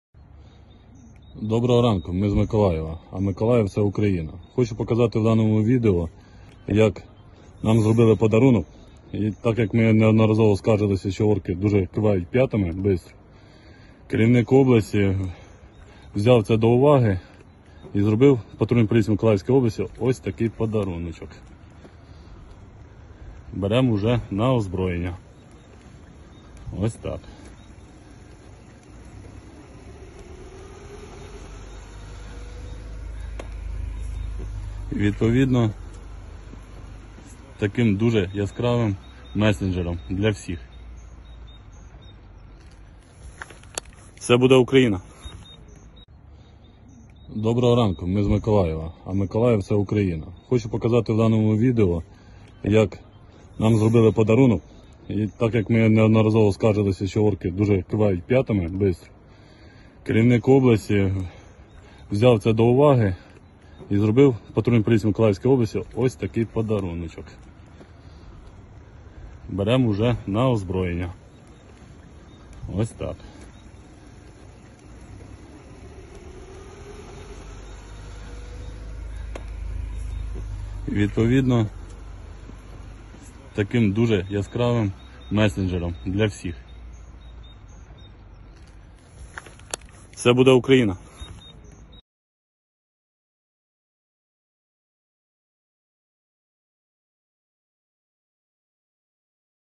BMW convertible fitted with machine gun spotted in Mykolaiv Ukraine amid Russian invasion. The World
A BMW convertible with a large machine gun mounted to its rear was seen in Mykolaiv, Ukraine, in footage shared on Monday by regional governor Vitali Kim.

Governor Kim described the car as a gift for the local police. A man speaking in the footage can be heard saying the vehicle was given to them by the regional administrative leader, a reference to Kim.

The video is among the latest shared by Kim on his active Telegram channel, where he regularly uploads videos promising defiance of Russian forces who are threatening the city.

Below The Page SEO.